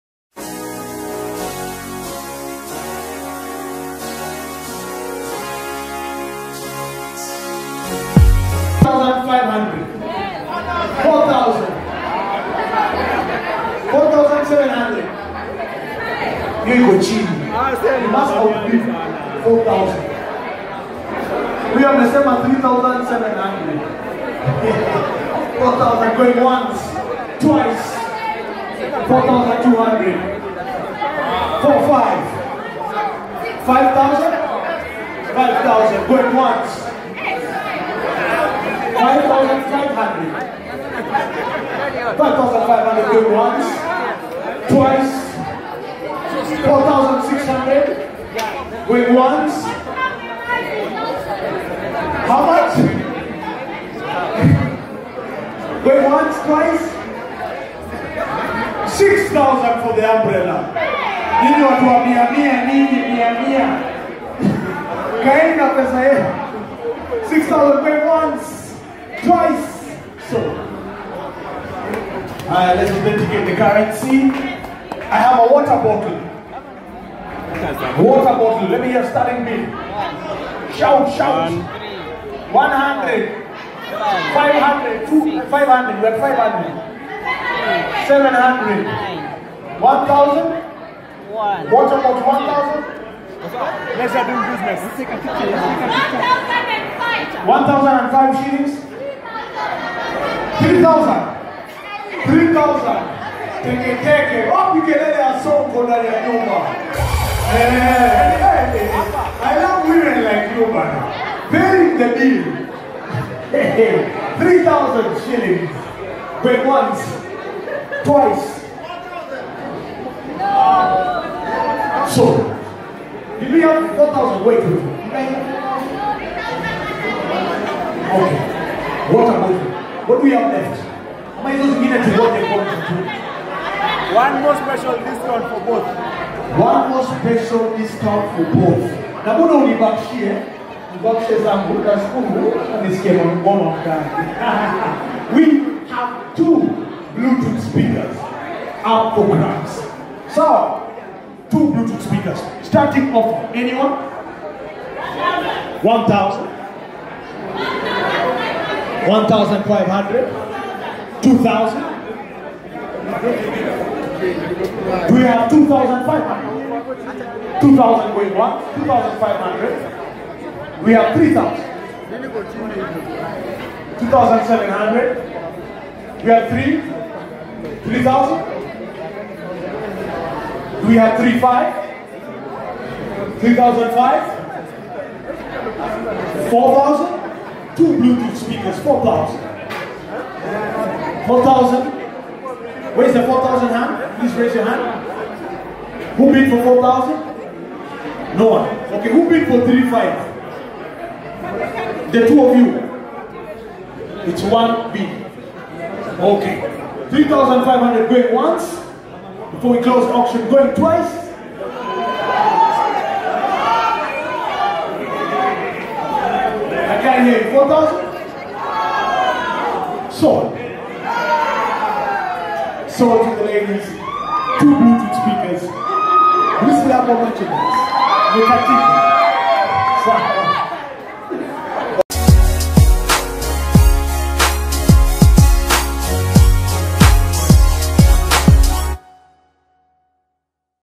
Break The Bank Part 2
BrandDNG introduces the Corporate Auction to NCBA Loop Customers!!!

Check out how much fun these VIPs had!

🎬 : 254 TV